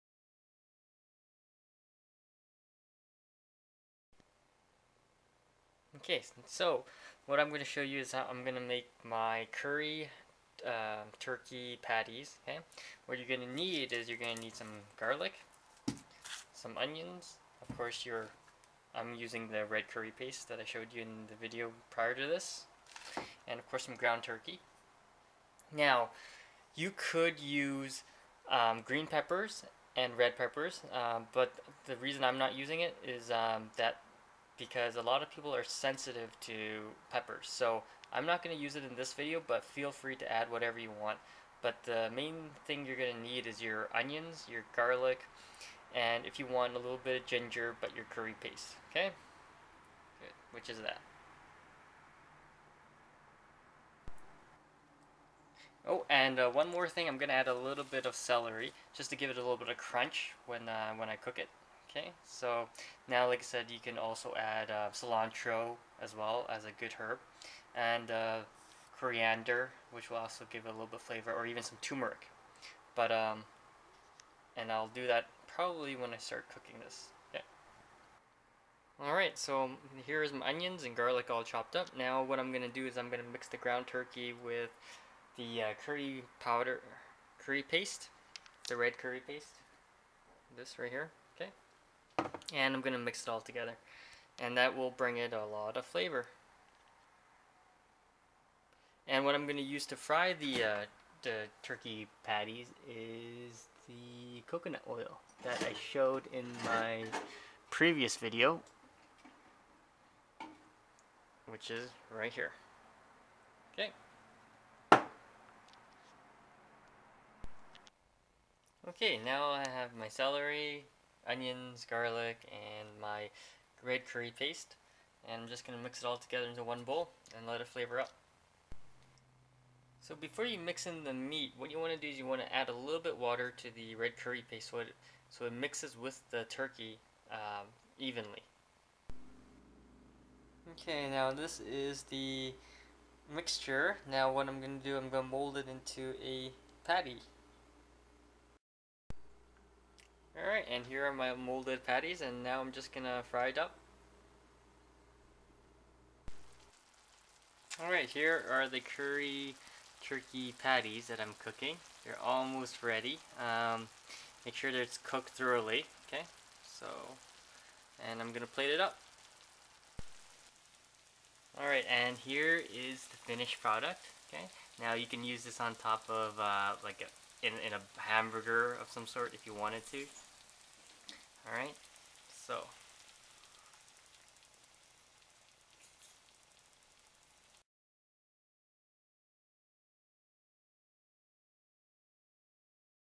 How to make healthy curry turkey patties hamburger recipe without red meat cow beef
This is a recipe on how to make healthy curry turkey patties, that you can use with hamburgers. It quite good but can take some time to make about 15 minutes for chop up the veggies. If you have any questions let me know.